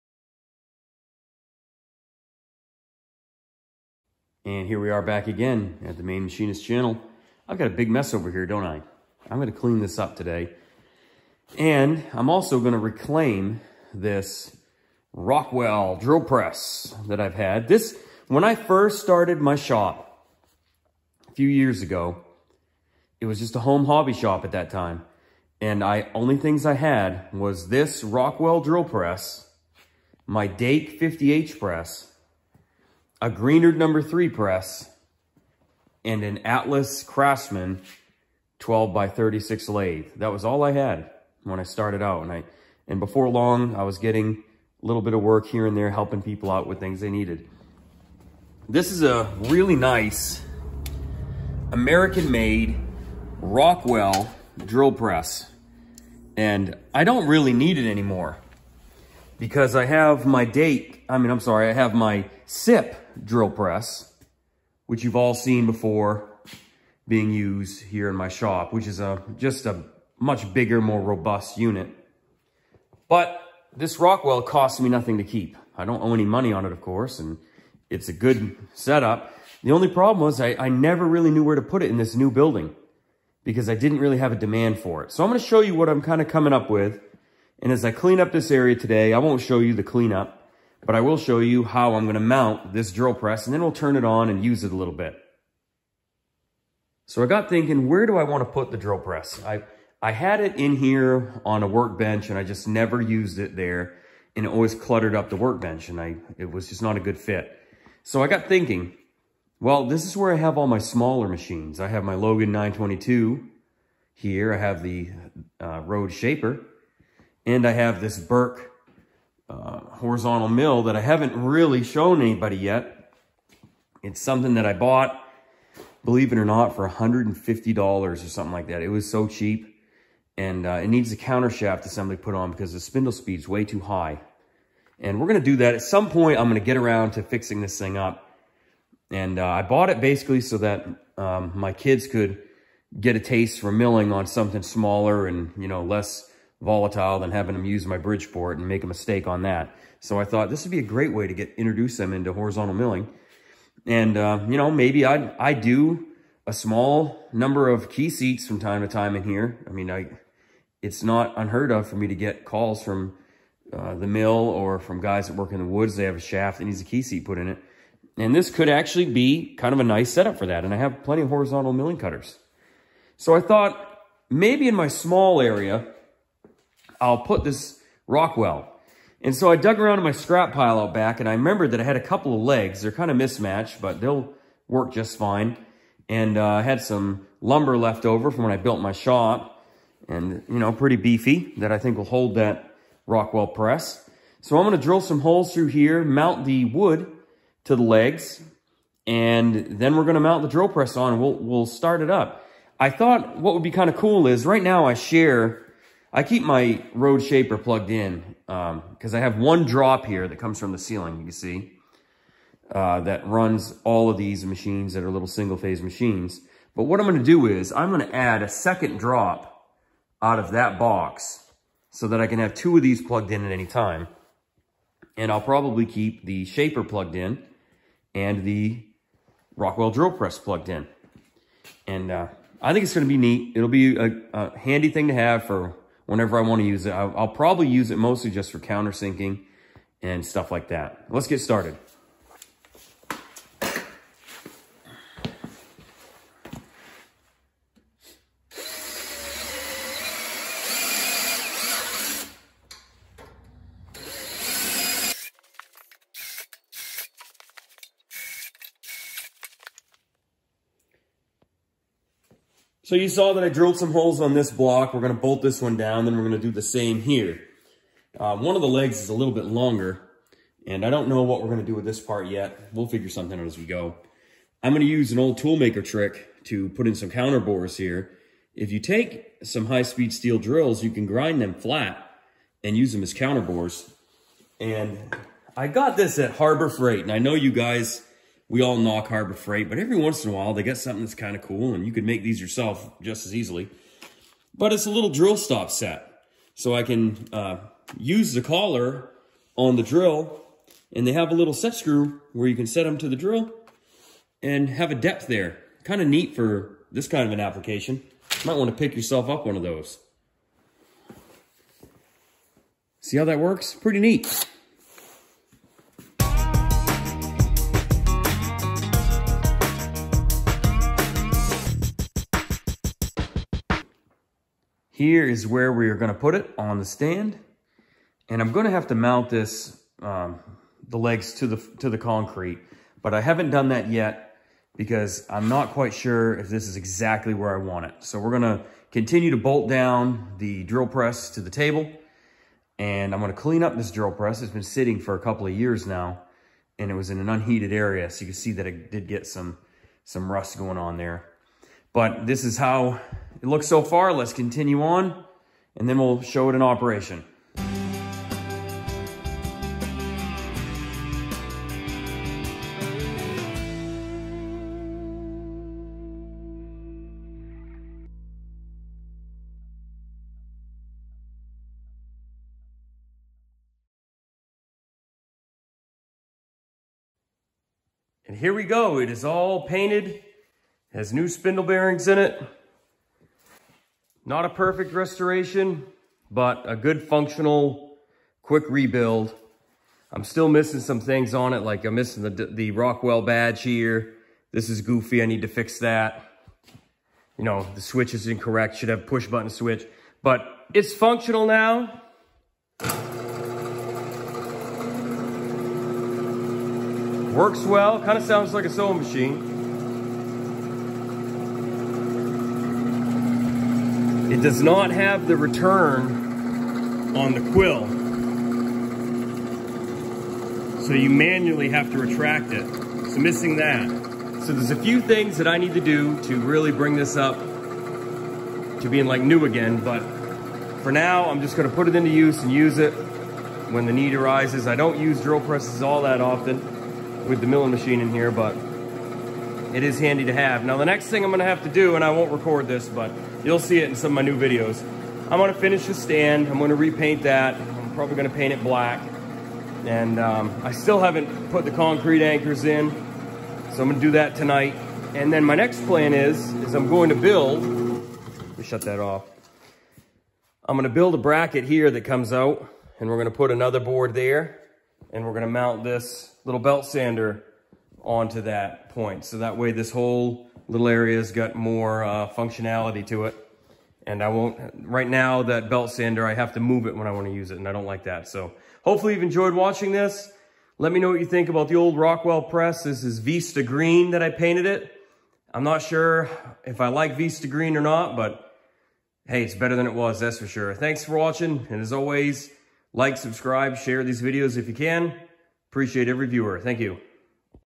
My First Drill Press - Vintage Rockwell USA
Bringing back my first Drill Press that I had in my home shop before opening my business. A vintage USA Rockwell Drill Press.

Chapped skin? Dry skin? Bug bites?